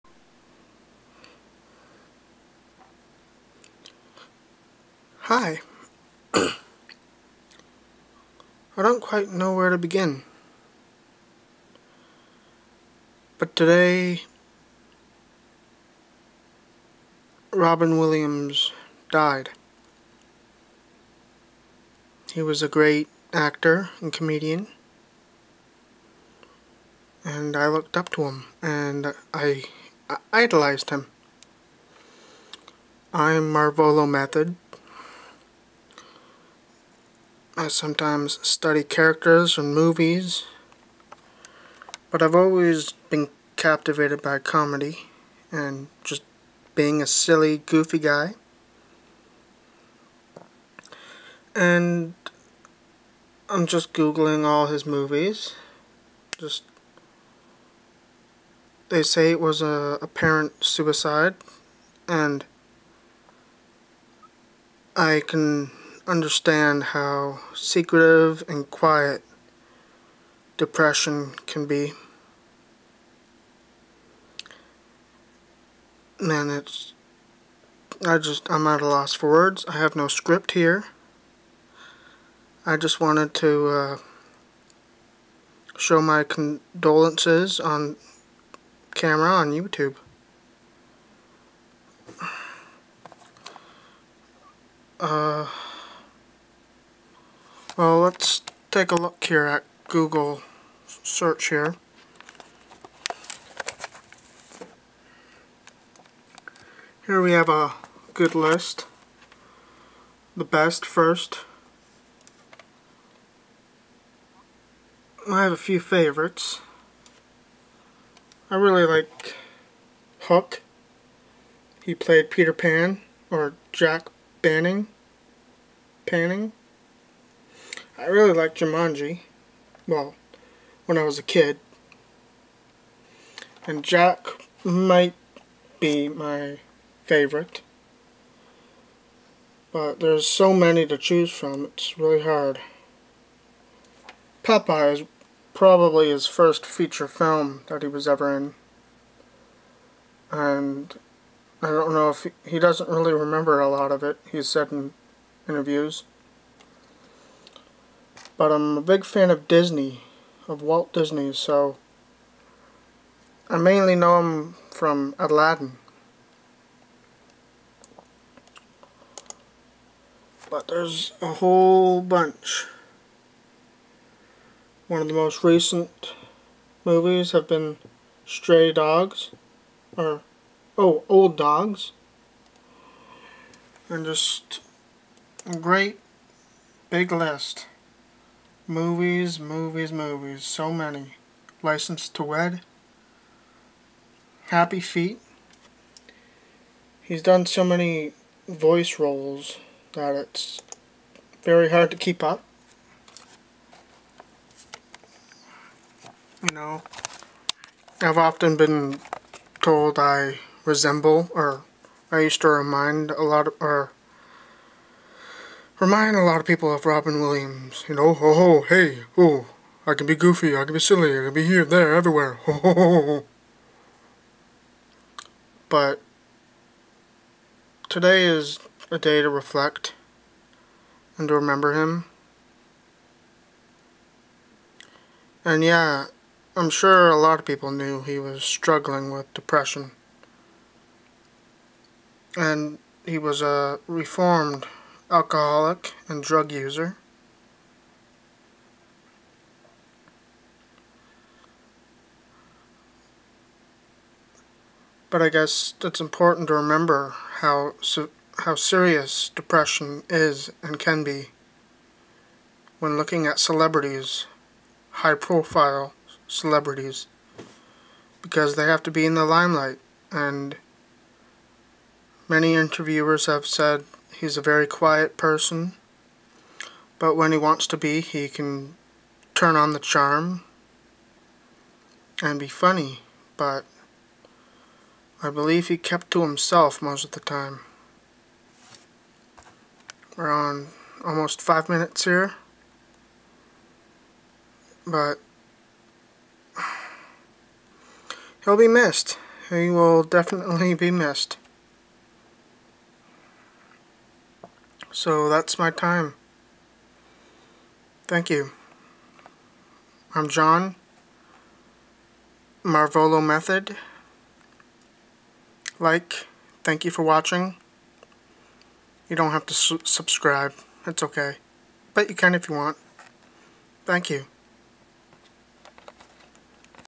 RIP Mr. Williams, you will be missed
monday august 11 2014 , actor and comedian Robyn Williams died,  a sad day for everyone who loved and admired his work, im not video genius, so this is a one take,  thank you for watching